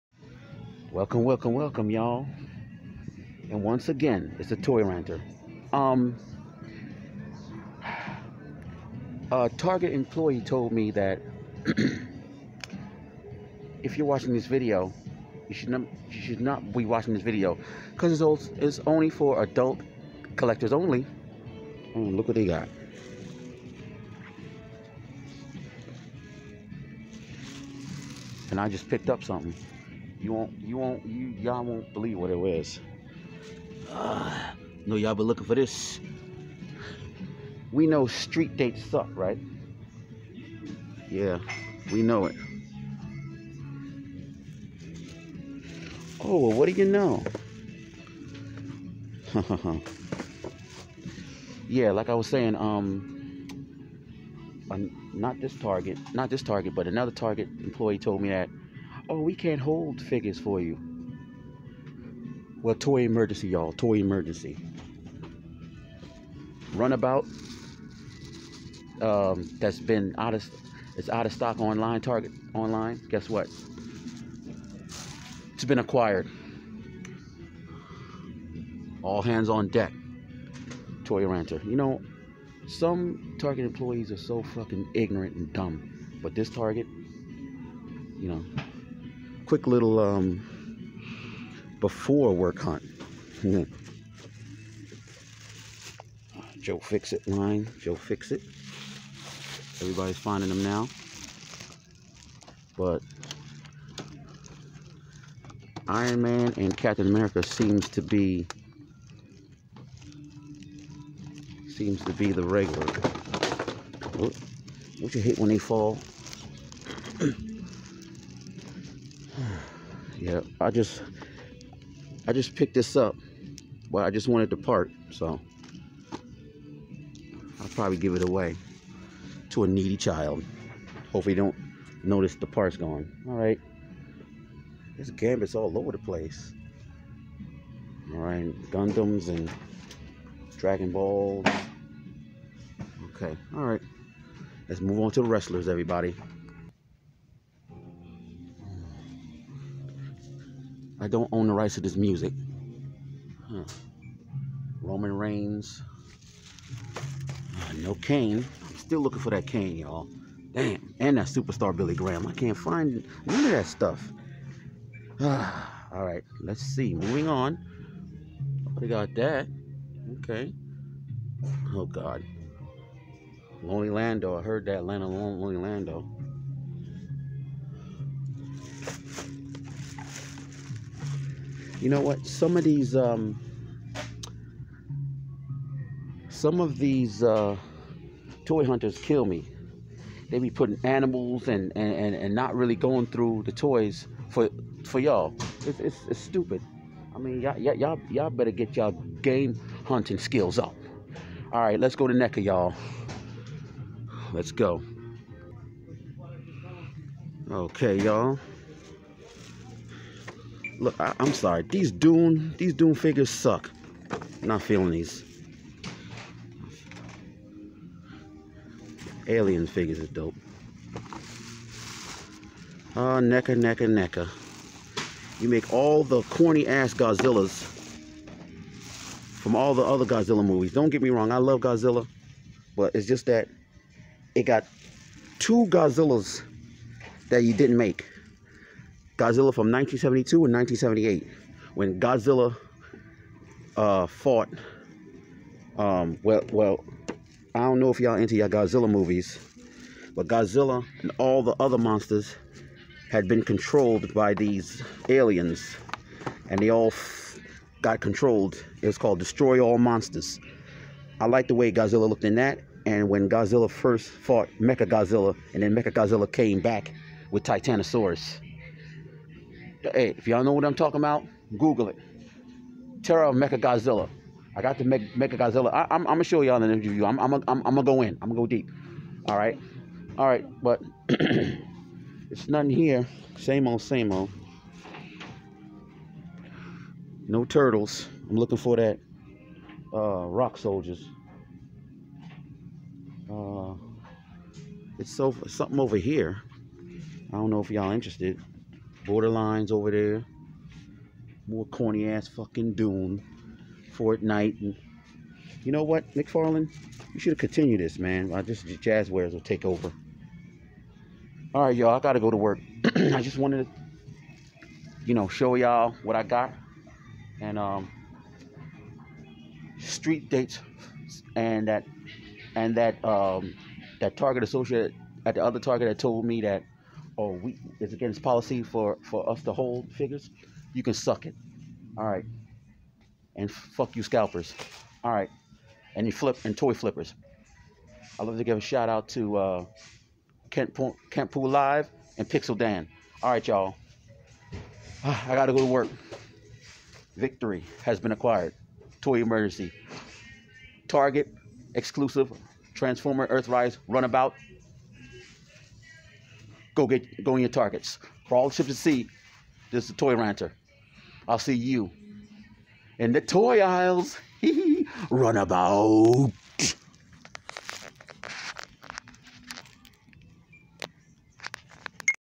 another toy emergency score!!! #2!!! Wit the toyranter
target toy Hunt! screw! street dates and policy that target can't hold some items for you!